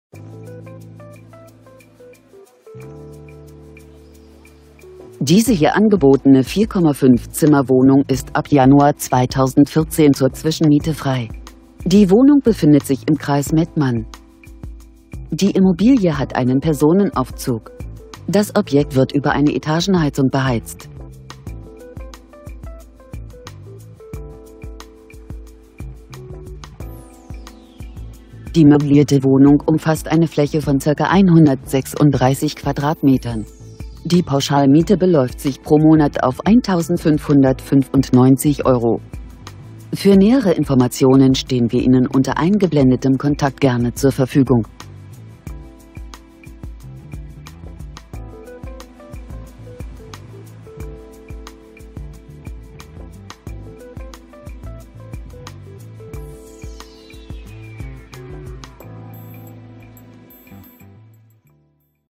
Ratingen-West: Helle 4,5-Zimmer-Wohnung mit großem Wohnzimmer mit Ledersofas und Zugang zu ei
Preis: 1.595€
Zimmer: 4,50
Wohnfläche: ca. 136m²

 Mitwohnzentrale